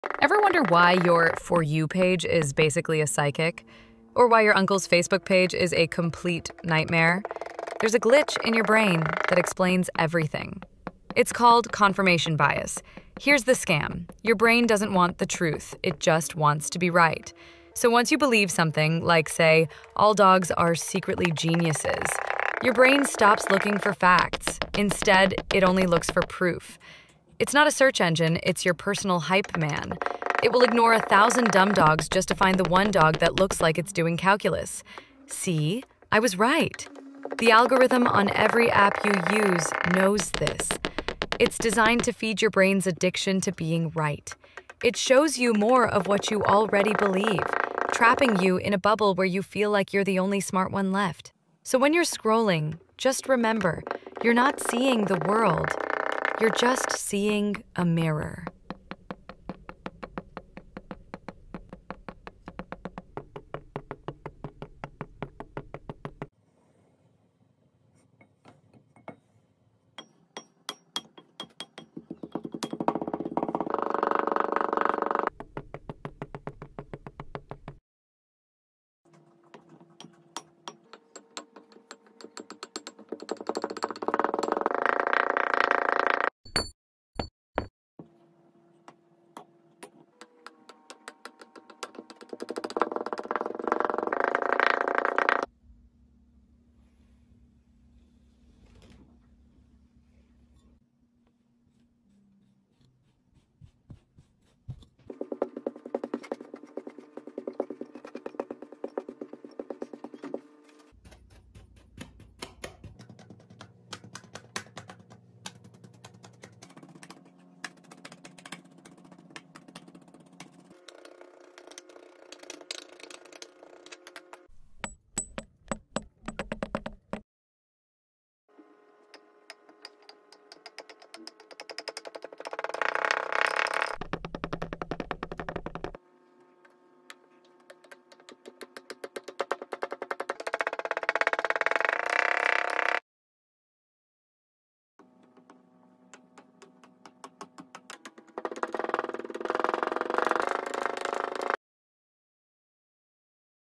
The Psychological Glitch Controlling Your Feed
Ever wonder why your social media feed feels like an echo chamber? It's because of a psychological glitch called Confirmation Bias. This cognitive bias tricks your brain into only seeing what it wants to believe, and the algorithm on platforms like TikTok and YouTube uses it to keep you scrolling.

In this video, we explain this complex psychology topic in a simple way.